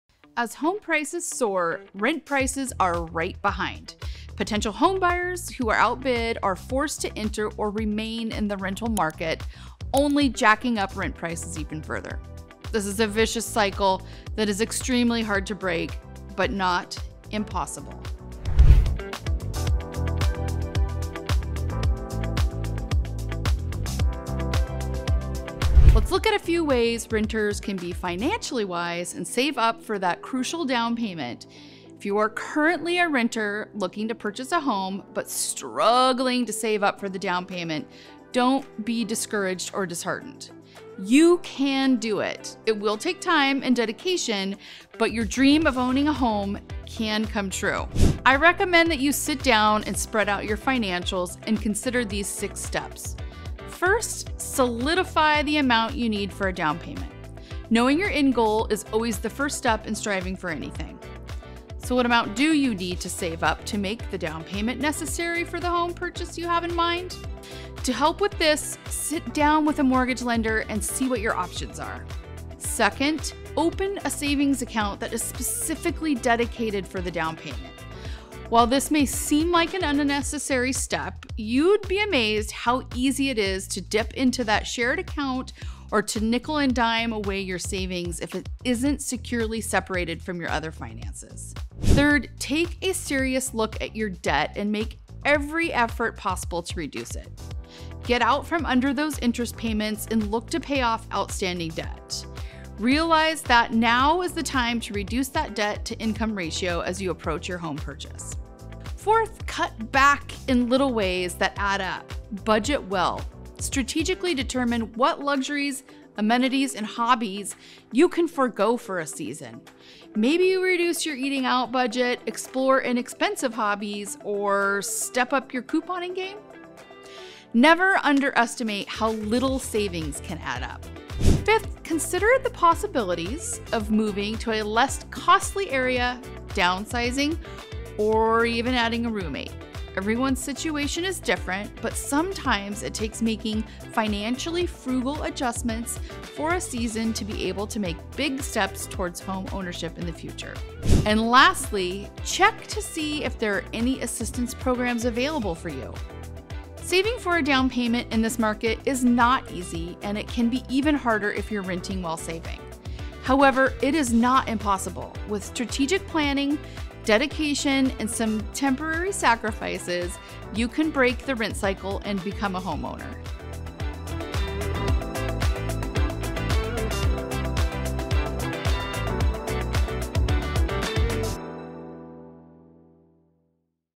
How to Save for a Down Payment While Renting | Jenell Steltz Real Estate
Do you dream of owning your own home? Are you stuck renting? This video is for you! Rent prices are at an all time high right now and it has many renters struggling to save for that down payment. If you are feeling incredibly disheartened about making practical steps towards building up your savings, you are not alone. But, don’t be too discouraged because owning your own home IS POSSIBLE! That’s right - you can do it! It will take hard work, dedication, sacrifice, and time, but your dream of being a homeowner can come true! Take a look at this week’s video for six tips for how to save while renting and get even closer to the ultimate goal of homeownership! Want to see what I’ve got listed in the area and start to envision the home you’re saving for?

As home prices sore, rent prices are right behind. Potential homebuyers who are outbid are forced to enter or remain in the rental market - only jacking up rent prices event further. This is a vicious cycle that is extremely hard to break - but, not impossible! Let’s look at a few ways renters can be financially wise and save up for that crucial down payment.

If you are currently a renter looking to purchase a home, but struggling to save up for the down payment, don’t be discouraged or disheartened. You CAN do it! It will take time and dedication, but your dream of owning a home can come true. I recommend that you  sit down, spread out your financials, and consider these six steps.

First, solidify the amount you need for a down payment. Knowing your end goal is always the first step in striving for anything. So, what amount do you need to save up to make the down payment necessary for the home purchase you have in mind? To help with this sit down with a mortgage lender and see what your options are.

Second, open a savings account that is specifically dedicated for the down payment. While this may SEEM like an unnecessary step, you’d be amazed how easy it is to dip into a shared account or to nickel and dime away your savings if it isn’t securely separated from your other finances.

Third, take a serious look at your debt and make every effort possible to reduce it. Get out from under those interest payments and look to pay off outstanding debt. Realize that now is the time to reduce that debt to income ratio as you approach your home purchase.

Forth, cut back in little ways that add up. Budget well! Strategically determine what luxuries, amenities, and hobbies you can forgo for a season. Maybe you reduce your eating out budget, explore inexpensive hobbies, or step up your couponing game. Never underestimate how little savings can add up!

Fifth, consider the possibilities of moving to a less costly area, downsizing, or adding a roommate. Everyone’s situation is different, but sometimes it takes making financially frugal adjustments for a season to be able to make big steps towards homeownership in the future.

And lastly, check to see if there are any assistance programs available for you.

Saving for a down payment in this market is not easy and it can be even harder if you are renting while saving. However, it is not impossible! With strategic planning, dedication, and some temporary sacrifices, you can break the rent cycle and become a homeowner!